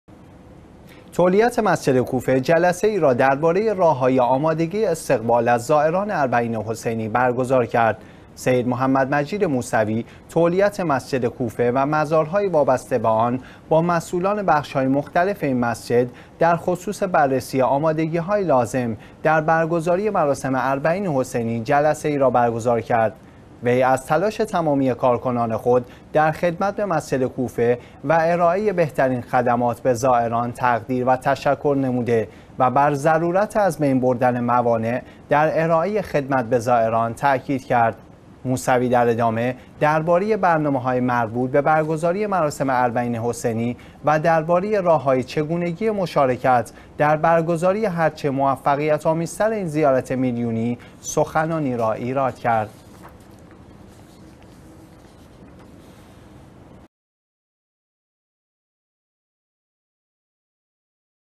جلسه تولیت مسجد کوفه برای استقبال از زائران اربعین حسینی
تولیت مسجد کوفه جلسه ای را درباره راه های آمادگی استقبال از زائران اربعین حسینی برگزار کرد.

 سید محمد مجید موسوی، تولیت مسجد کوفه و مزارهای وابسته به آن با مسئولان بخش های مختلف این مسجد در خصوص بررسی آمادگی های لازم در برگزاری مراسم اربعین حسینی جلسه ای را برگزار کرد.
وی از تلاش تمامی کارکنان خود در خدمت به مسجد کوفه و ارائه بهترین خدمات به زائران تقدیر و تشکر نموده و بر ضرورت از بین بردن موانع در ارائه خدمت به زائران تاکید کرد.
موسوی در ادامه درباره برنامه های مربوط به برگزاری مراسم اربعین حسینی و درباره راه های چگونگی مشارکت در برگزاری هر چه موفقیت آمیزتر این زیارت میلیونی سخنانی را ایراد کرد.